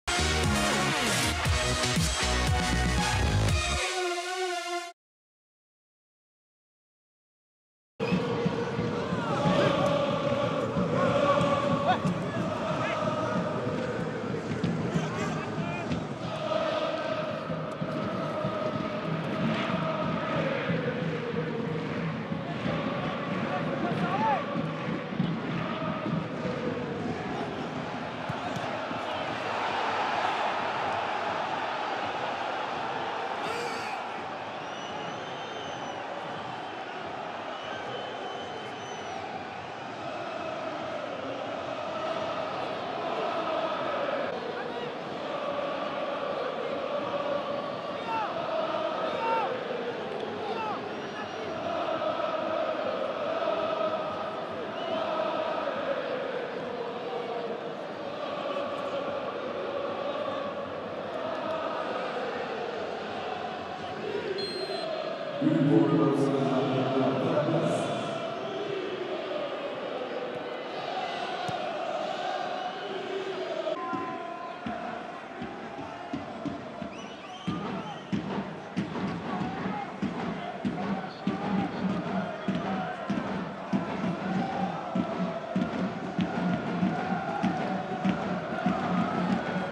But Jonathan DELAPLACE (14') / FC Nantes - LOSC Lille (1-1) - (FCN - LOSC) / 2014-15
FC Nantes - LOSC Lille (1-1) - But de Jonathan DELAPLACE (14'). Tous les buts de FC Nantes - LOSC Lille en vidéo.

Stade de la Beaujoire-Louis Fonteneau - samedi 31 janvier 2015

Buteurs : Jonathan DELAPLACE (14' - LOSC Lille) / Oswaldo VIZCARRONDO (80' - FC Nantes)